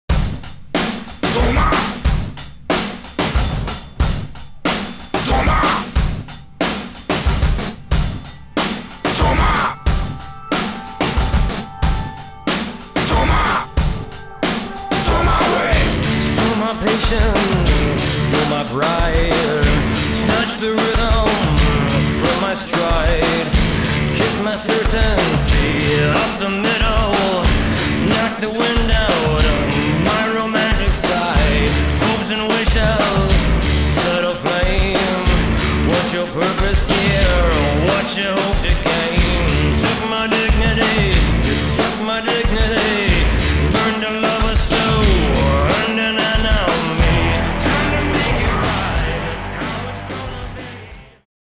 Maynard James Keenan's Puscifer Behind the Scenes Teaser
Maynard James Keenan's Puscifer warm up for upcoming 2013 tour dates